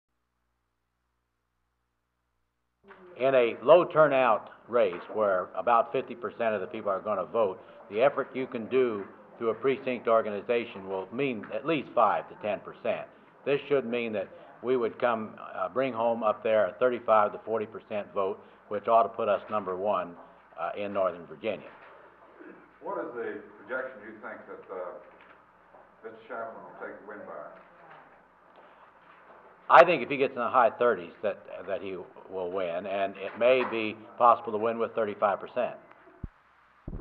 10/27/71 Interview with 10th District Republican Chairman William Stanhagan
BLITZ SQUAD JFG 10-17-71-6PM
 THE REPUBLICAN VICTORY BLITZ SQUAD WAS IN ROANOKE AS PART OF THREE DAY TRIP ACROSS THE STATE TO DRUM UP SUPPORT FOR THE REPUBLICAN CANDIDATE FOR LIUTENAT GOVERNOR GEORGE SHAFRAN.
 TENTH DISTRICT REPUBLICAN CONGRESSIONAL CHAIRMAN WILLIAM STANHAGEN SAYS THAT SHAFRAN WILL DO WELL IN THE NOTHERN VIRGINIA AREA.
TIME.
SOF. MAG. OUT CUE.